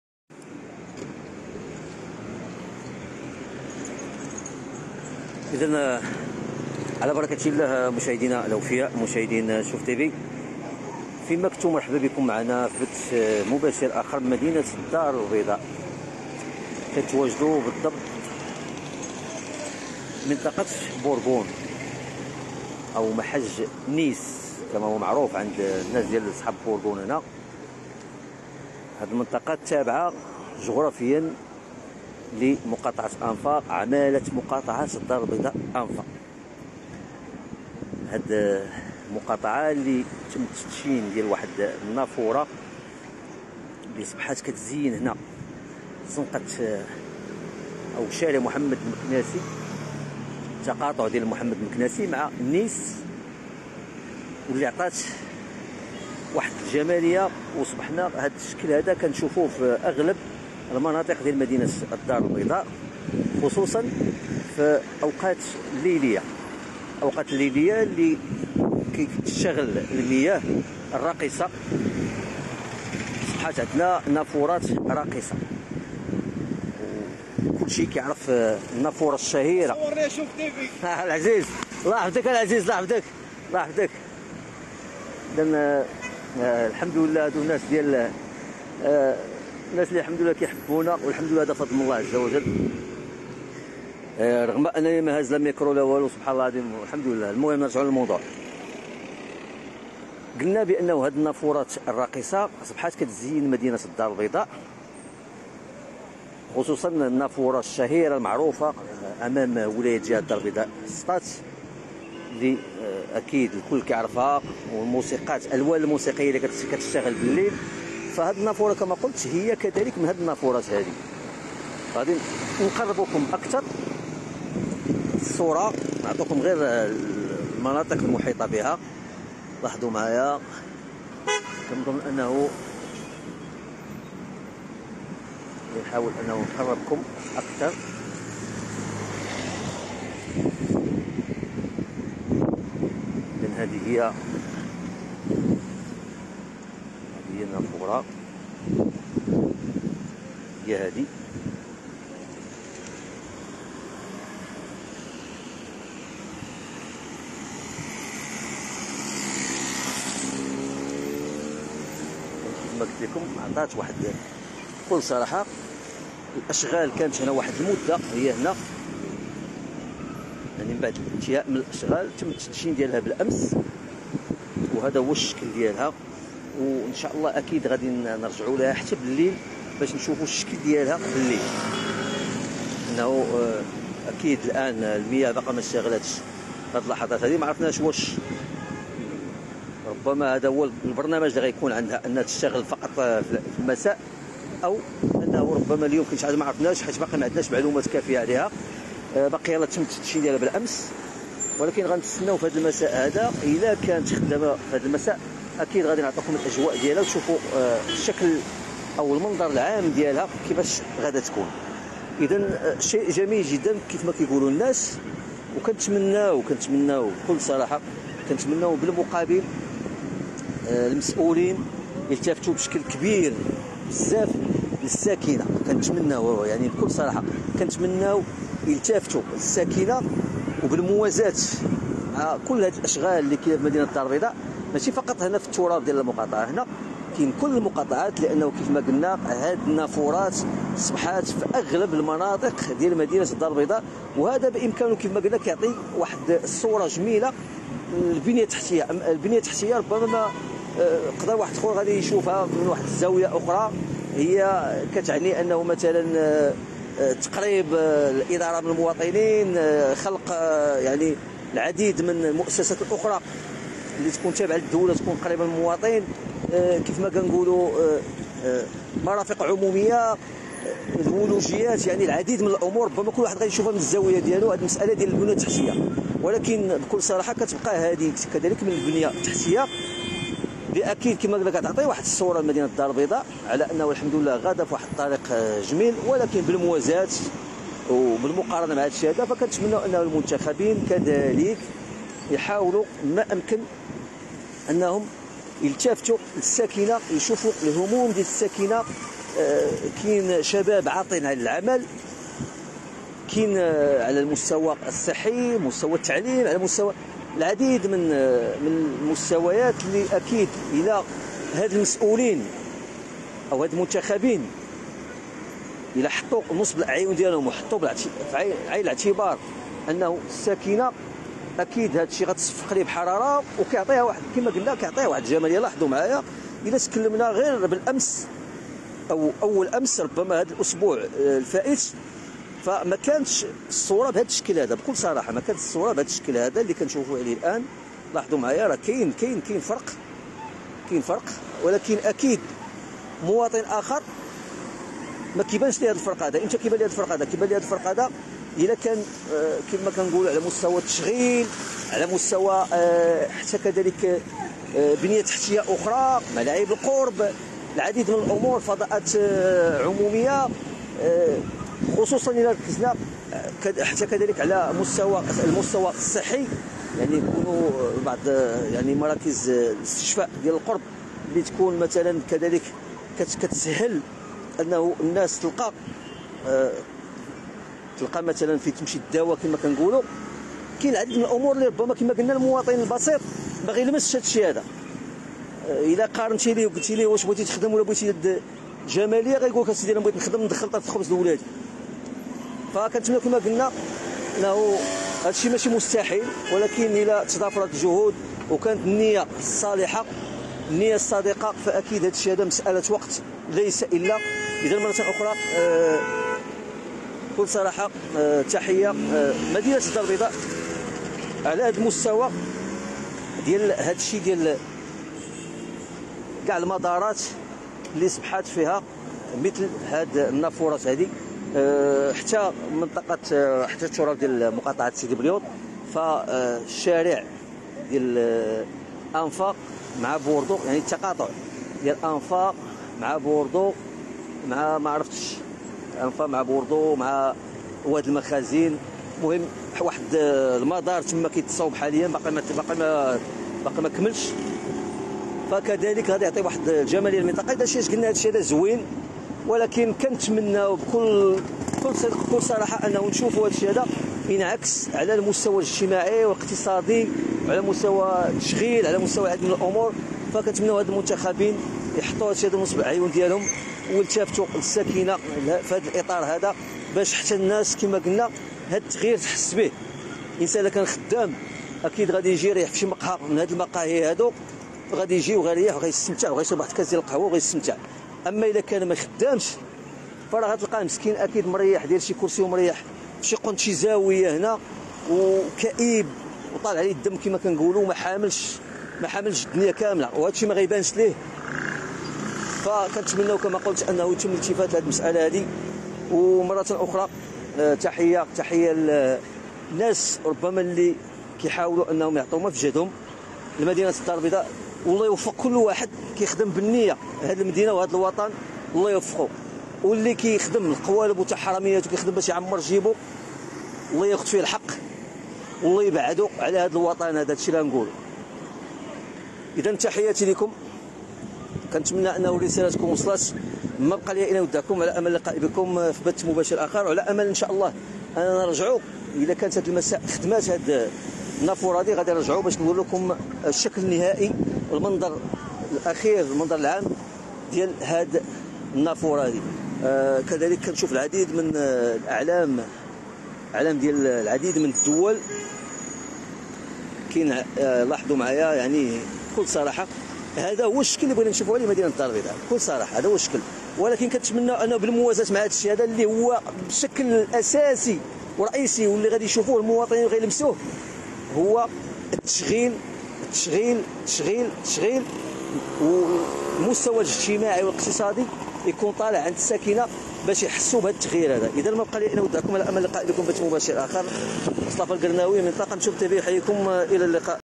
من الدار البيضاء : نافورة تم تدشينها بالأمس بتراب مقاطعة أنفا بمنطقة بوركون أعطت جمالية للمنطقة
مباشرة من الدار البيضاء : نافورة تم تدشينها بالأمس بتراب مقاطعة أنفا بمنطقة بوركون أعطت جمالية للمنطقة